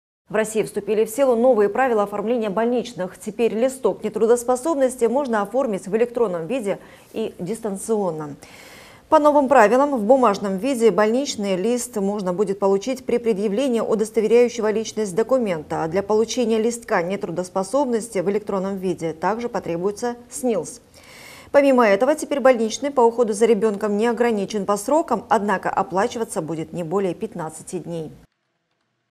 В России вступили в силу новые правила оформления больничных
В России вступили в силу новые правила оформления больничных. Теперь листок нетрудоспособности можно оформить в электронном виде и дистанционно.

По новым правилам, в бумажном виде больничный лист можно будет получить при предъявлении удостоверяющего личность документа, а для получения листка нетрудоспособности в электронном виде также потребуется СНИЛС.

Помимо этого, теперь больничный по уходу за ребёнком не ограничен по срокам, однако оплачиваться будет не более 15 дней.